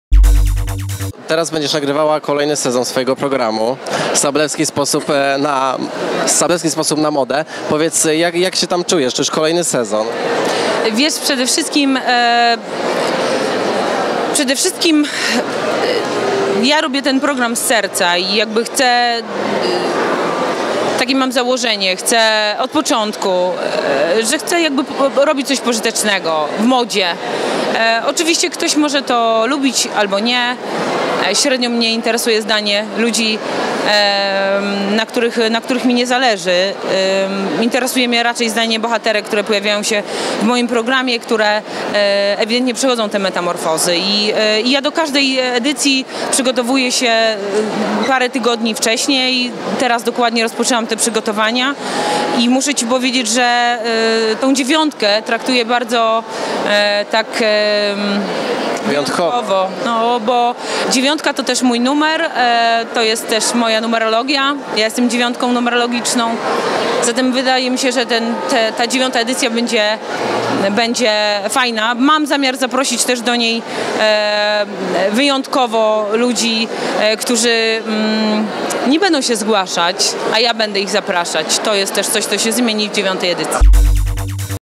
Maja Sablewska zdradza co nowego w IX edycji programu "Sablewskiej sposób na modę" | przeAmbitni.pl
First Lady - pokaz Macieja Zienia - 12.06.17 r.